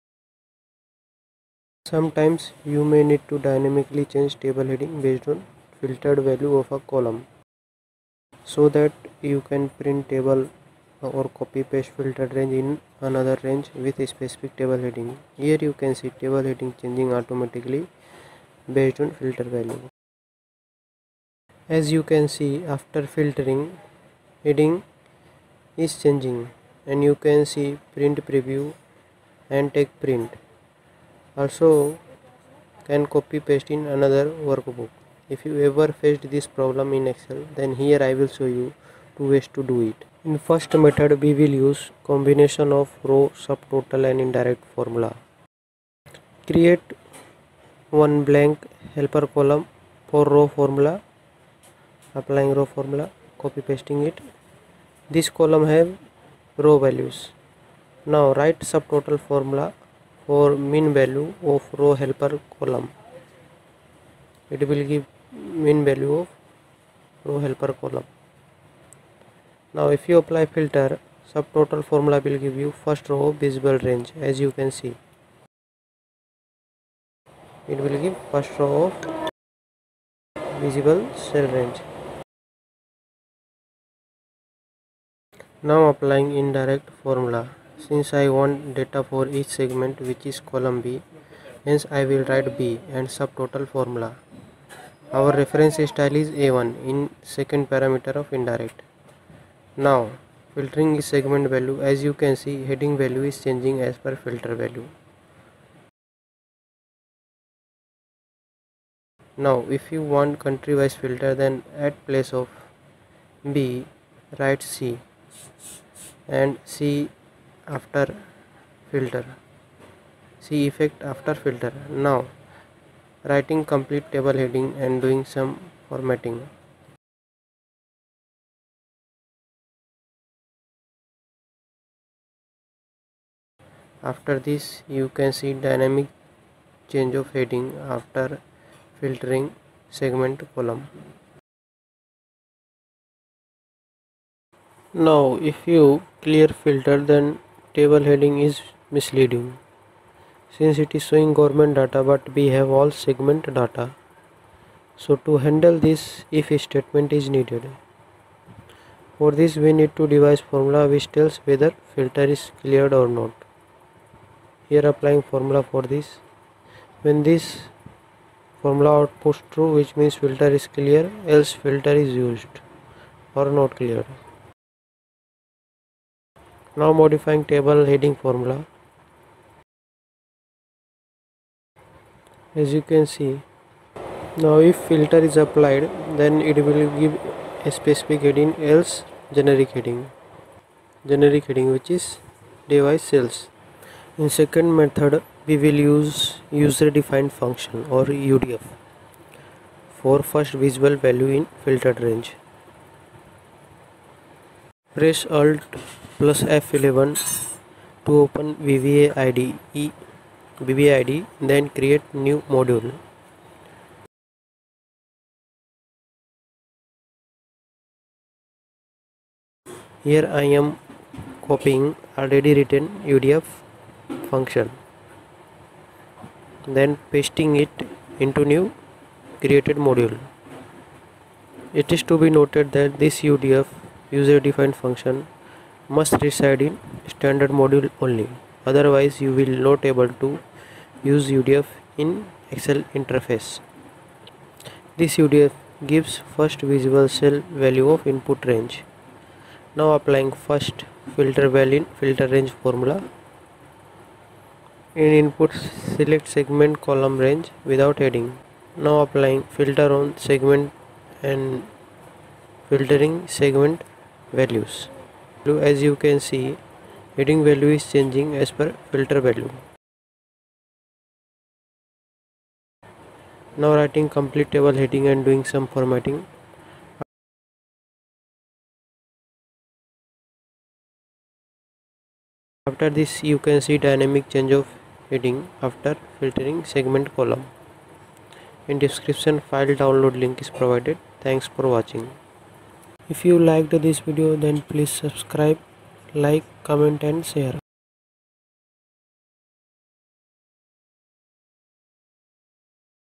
Get value from the first visible cell in filtered range | auto change table heading after filter
Text version of the topic

Have you ever wondered how to get the first visible cell value of filtered range or dynamically changing table header after applying filter in Excel then this video is for you.

Sometimes you may need to dynamically change table heading based on filtered value of a column.
So that you can print table or copy-paste filtered range in another range with specific table heading.
In this video, you will see table heading changing automatically based on filter value.

I will explain two ways of doing this.
In the 1st method, we will use combination of row, subtotal and indirect formula.
In the 2nd method, we will use user defined function or UDF for first visible cell value in filtered range.

Formula method - Get value from a first visible cell in filtered range 0:41
Formula to know filter is used or not 2:38
UDF method - Get value from first visible cell in filtered range 3:23

 🌐 Websites 🌐
📌📌You can watch and learn from the below videos also.📌📌